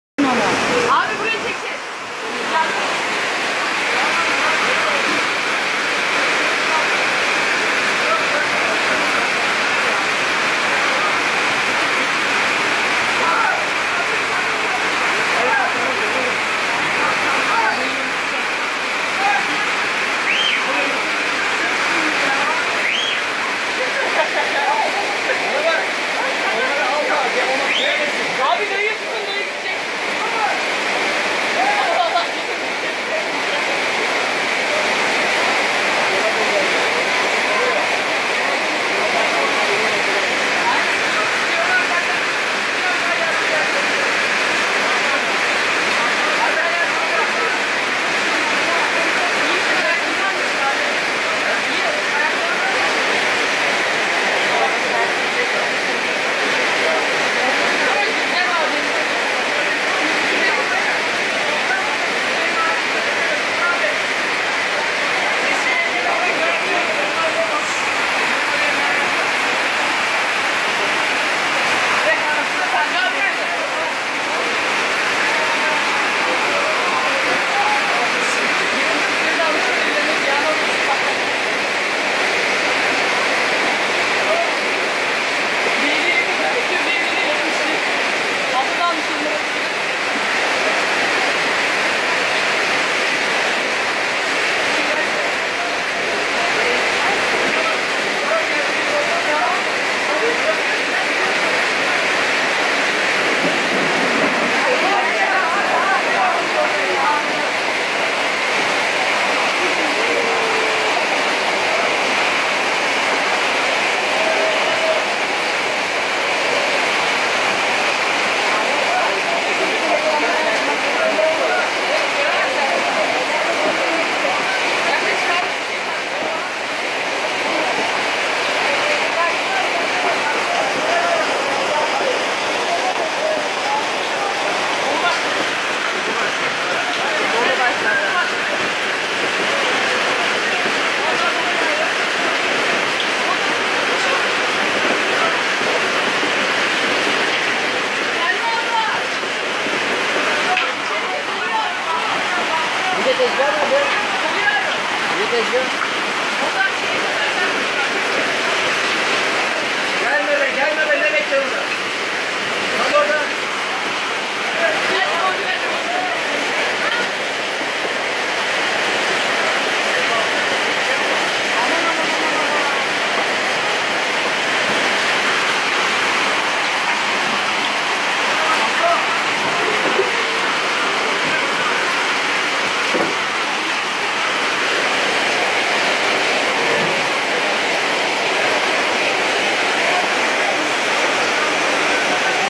Çanakkale Gelibolu'da Sel Felaketi ! | 26 06 2018
Akşam saatlerinde başlayan şiddetli yağmur Gelibolu'yu Çanakkale Boğazına çevirdi !
Major Flood in Gallipoli, Canakkale, Turkey